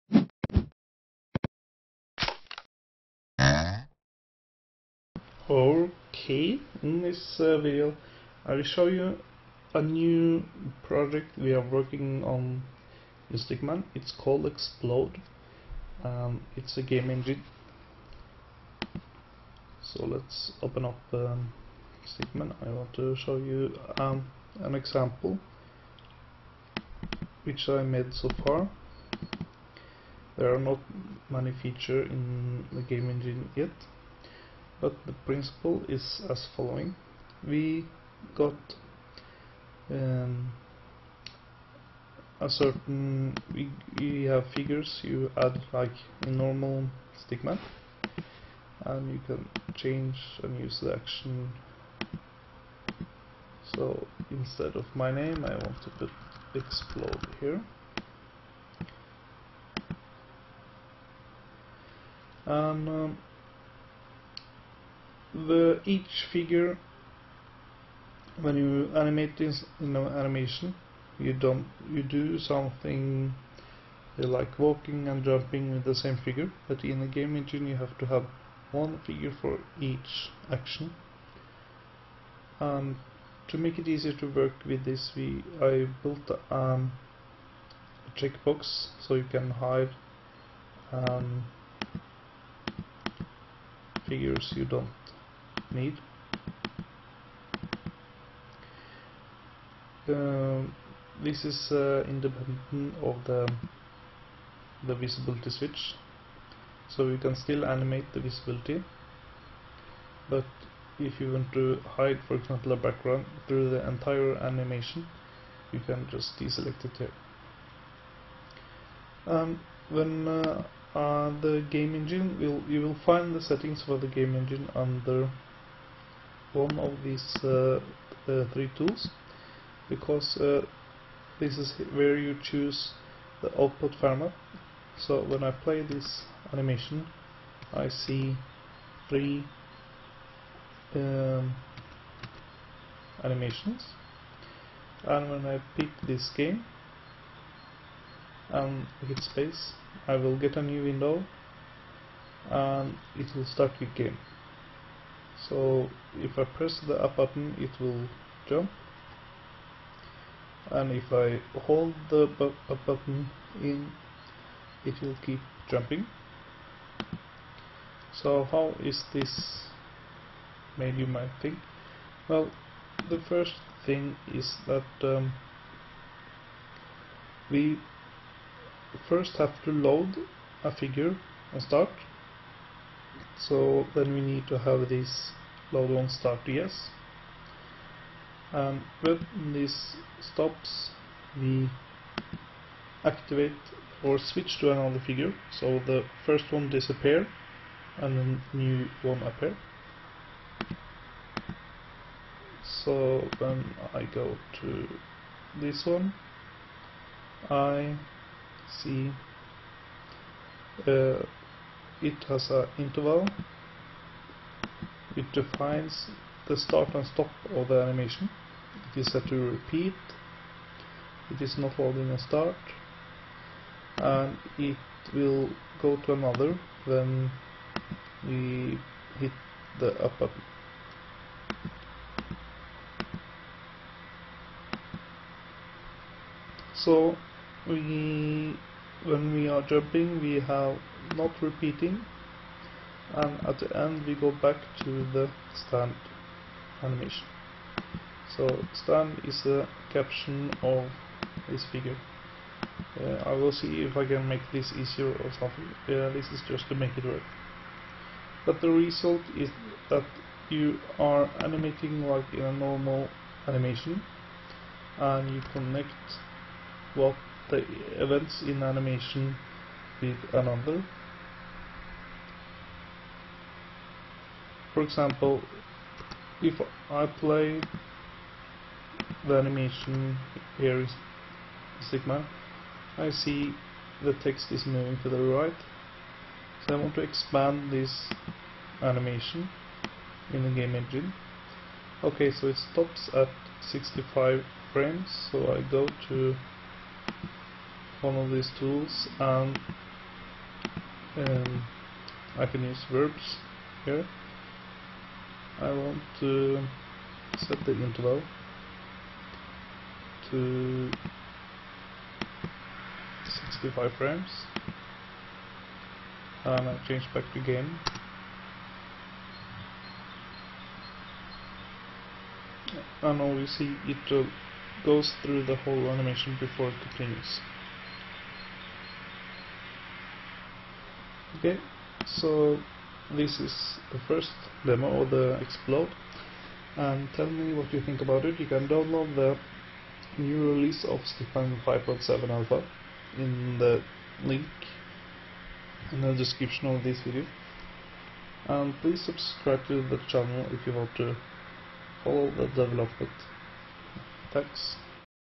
5.7 Part 13 - Xplode Game Engine
In this game engine, the philosophy is to focus on the animations and not so much on the logic. I want to make it as easy to create a game as it is to animate with Stickman.

The game engine uses a stickman animation as framework. It works in the same way, only with non-linear time. The plan is to add physics and location-specific events. For example, running on walls in a fighting game and collision+damage detection when hitting another figure.

A good game depends on how challenging and difficult it is. I think that to find out how much points you should give for a skill in order to make the game great will take too much time for almost everybody. Therefore, my idea is to make Stickman analyze the game when you play, and calculate the values automatically. Then you can switch between hard, medium and easy.

To make this possible I need to make a standard of life, health and so on. When the player start a new game, he will have:
Strength 100
HP 100
Life 3
Magic 100
Shield 100
Resistance 100
and other values.

The player will advance in levels which will increase in difficulty, require more points at higher levels.

Because Stickman will adjust the difficulty automatically, it will be possible to combine your scenes with others to create a bigger game. For example in a school class each student can create their own level and the class can then combine them into a game, for example with 20 levels.